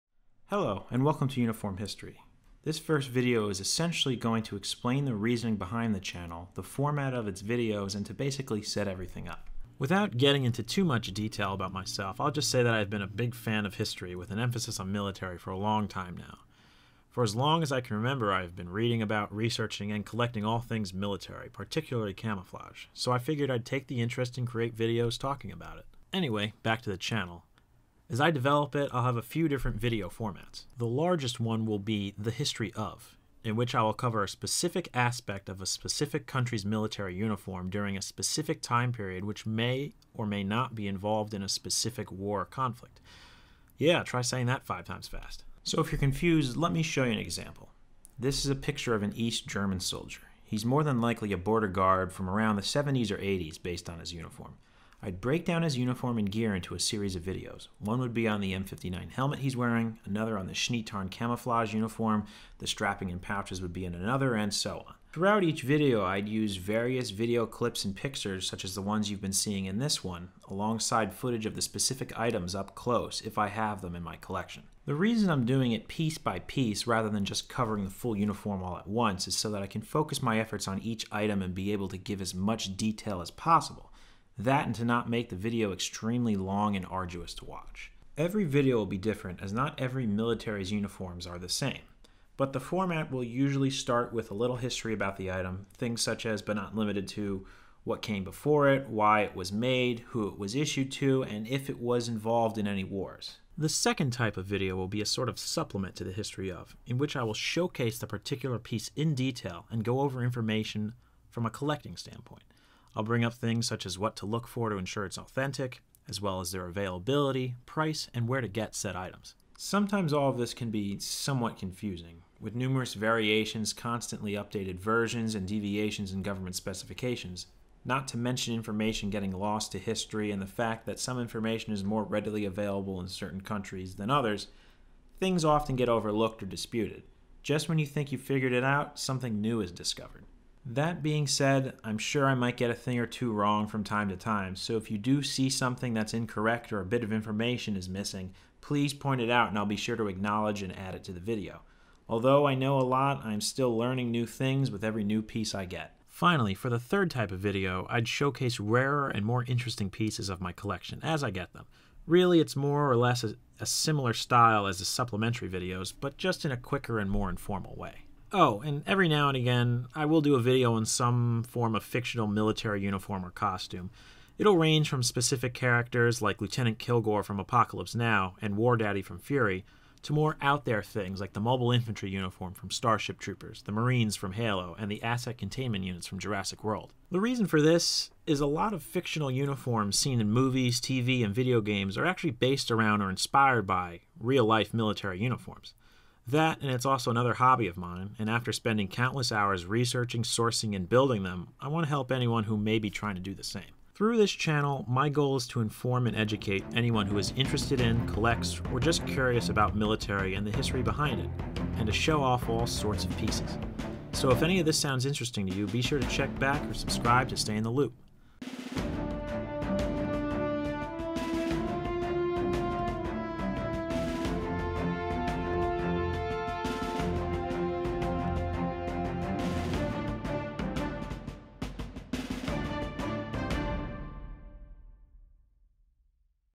Introductions | Uniform History
An introductory video in which we introduce ourselves, go over the format of videos as well as the general goals of the channel.